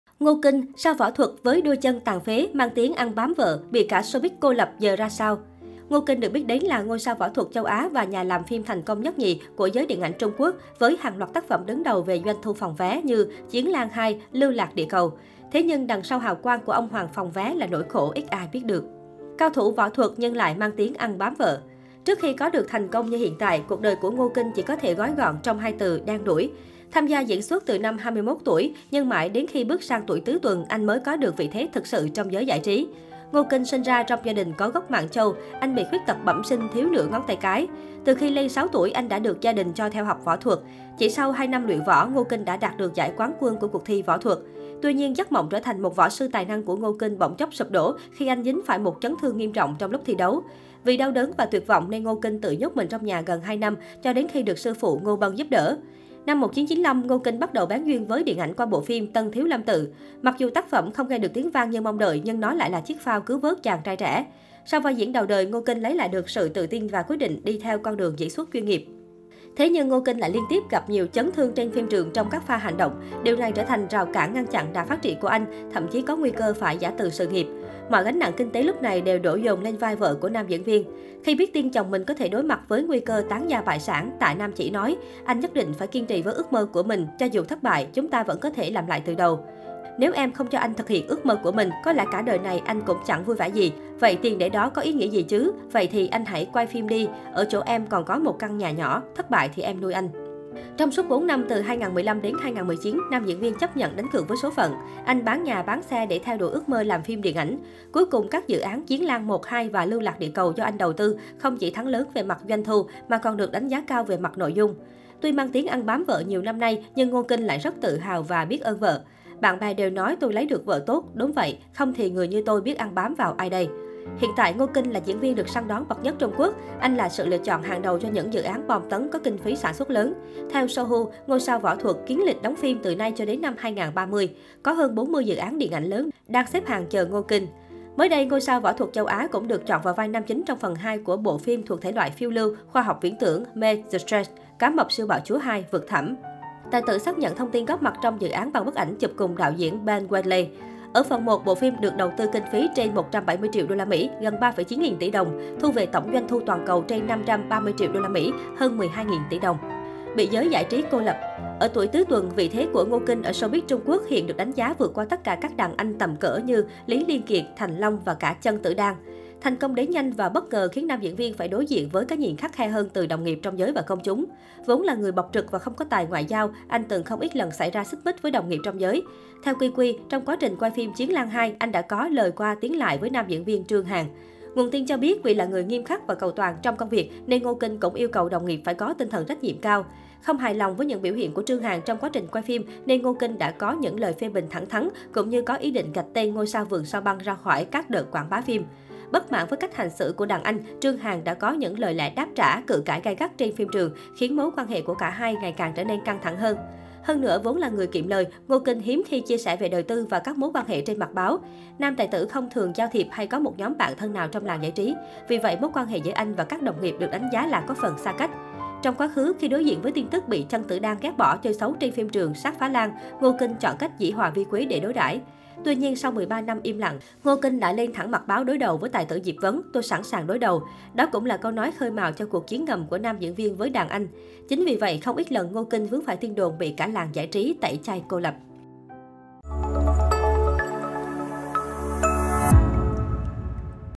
Ngô Kinh - Sao võ thuật với đôi chân tàn phế, mang tiếng ăn bám vợ, bị cả showbiz cô lập giờ ra sao?
Ngô Kinh được biết đến là ngôi sao võ thuật Châu Á và nhà làm phim thành công nhất nhì của giới điện ảnh Trung Quốc với hàng loạt tác phẩm đứng đầu về doanh thu phòng vé, như Chiến Lang 2, Lưu Lạc Địa Cầu... Thế nhưng, đằng sau hào quang của “ông hoàng phòng vé” là nỗi khổ ít ai biết được.